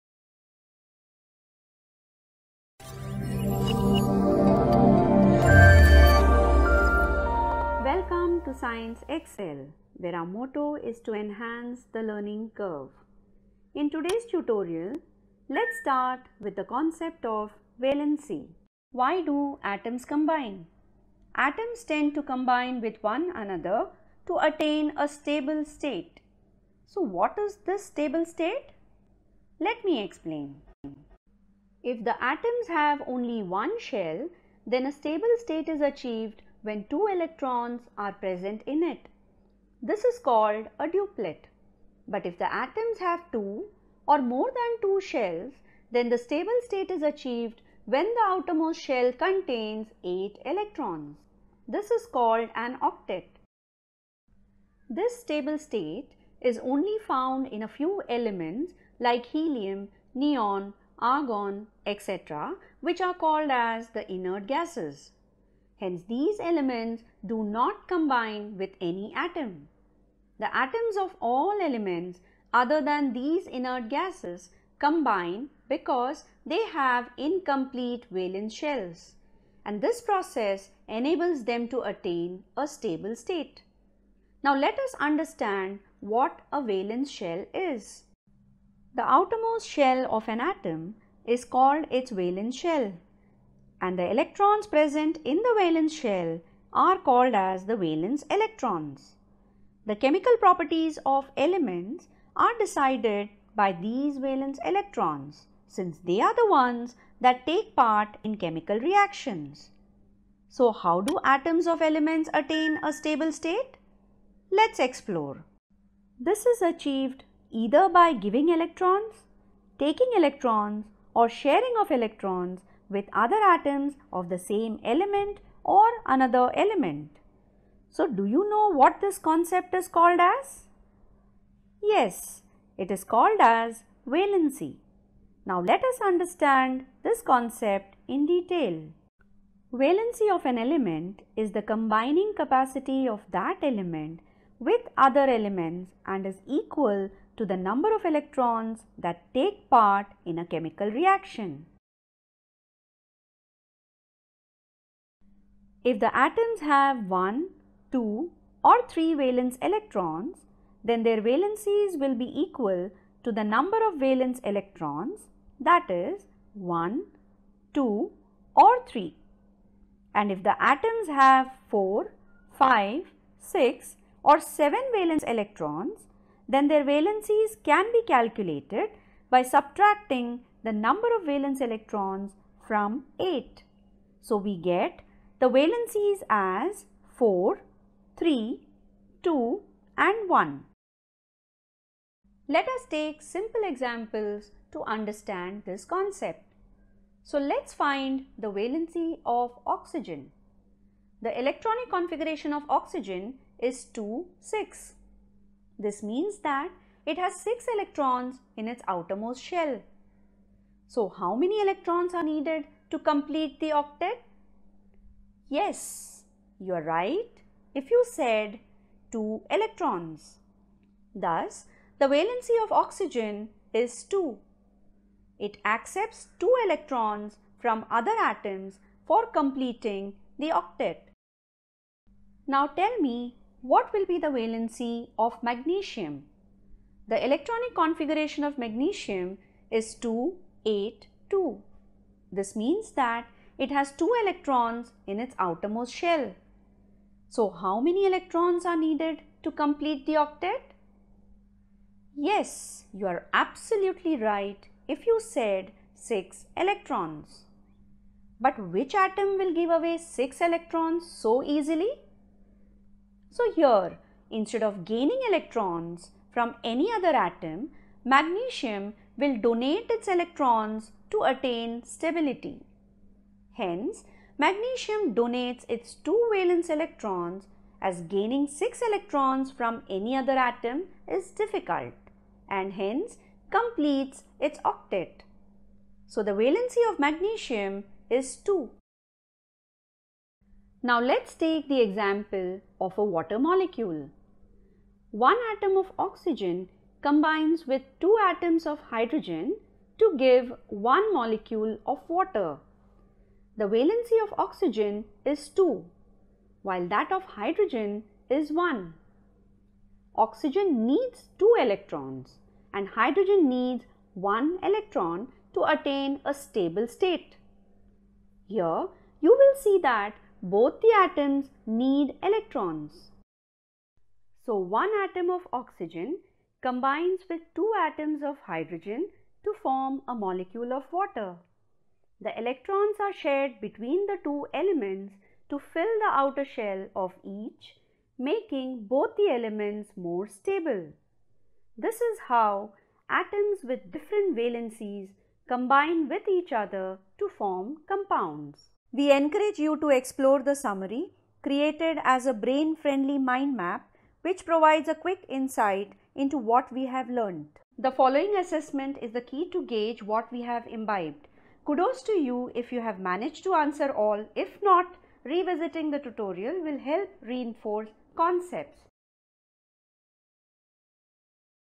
Valency | Science Excel
We at Science excel present this tutorial which provides an insight into the concept of valency.
The topic sheds light on valency and electronic configuration
Team Science Excel